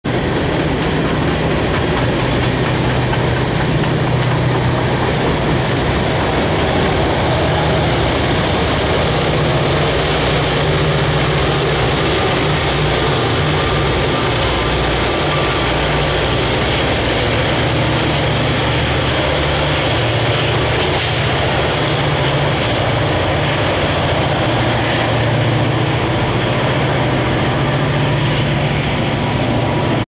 Norfolk Southern SD40E Helpers Working Hard, Altoona PA
4/23/16 This is just a quick video of a pair of NS SD40E Helper Units doing their thing, Working hard. Altoona Amtrak Station.
Helper Unit #'s NS 6304 & NS 6303